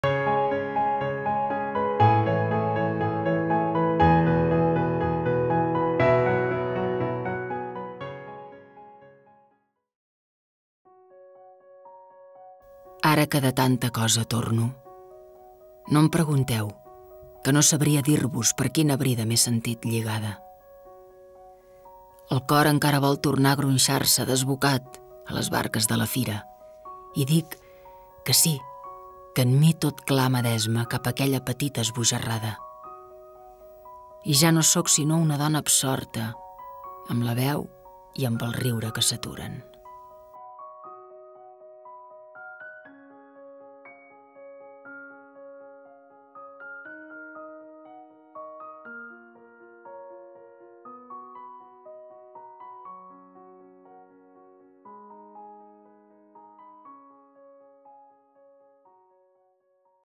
"Ara", de Clementina Arderiu [projecte Poesia Dibuixada]
Impulsat per celebrar els 15 anys de LletrA (Universitat Oberta de Catalunya) com una col·laboració de la UOC i Catorze, POESIA DIBUIXADA és un projecte literari i multimèdia on l’art, la poesia i l’audiovisual es fusionen per oferir una experiència lectora innovadora a partir de la creació de breus obres d’art audiovisuals. Cadascuna de les poesies dibuixades integra un poema d’un autor o autora clàssic o contemporani, la veu de Sílvia Bel i les il·lustracions de Marta Bellvehí, que hi posa el color amb pinzellades d’aquarel·la i línies a llapis.

PoesiaDibuixada amb un poema de Clementina Arderiu